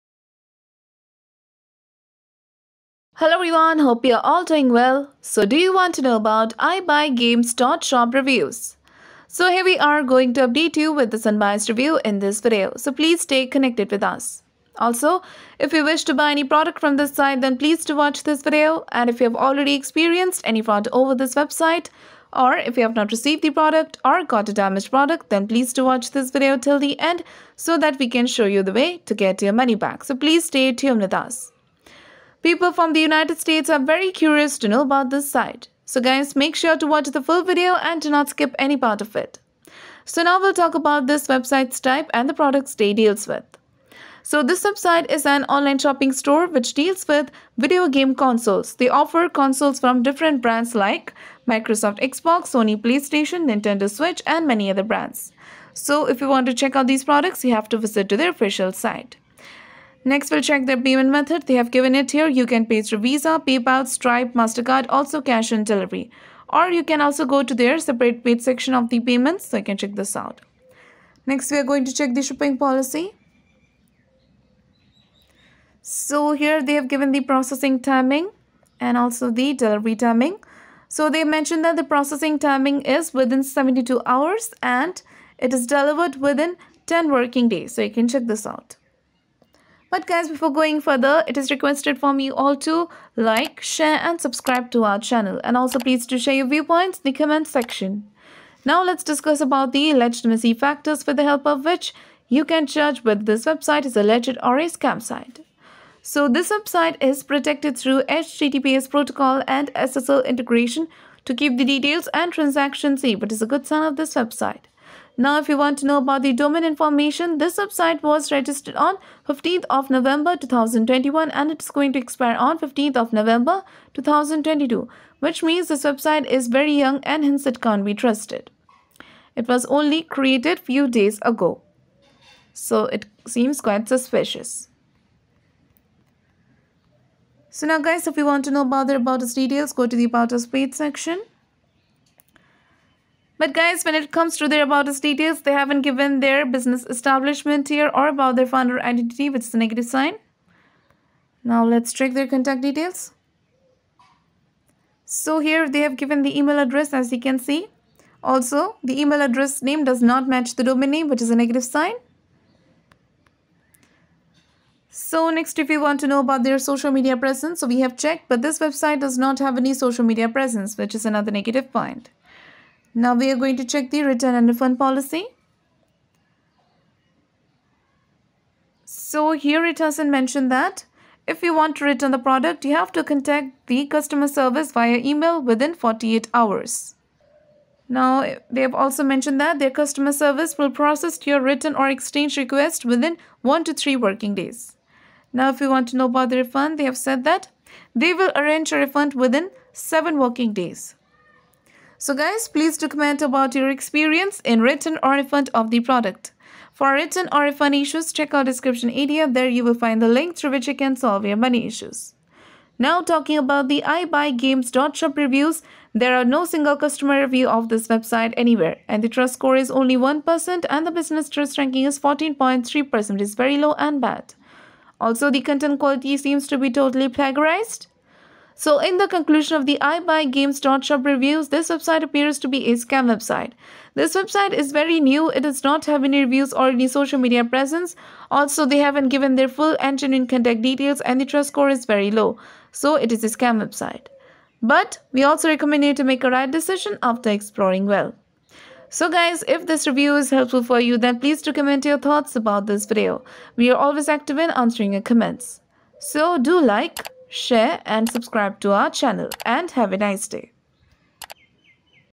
Ibuygames.shop Reviews (Nov 2021) - Is This A Trustworthy Site? Check It! | Scam Inspecter
Do you want to know about Ibuygames.shop Reviews ?  Watch this video till end because in this video you will get to know about the legitimacy of the site.

Scam Inspecter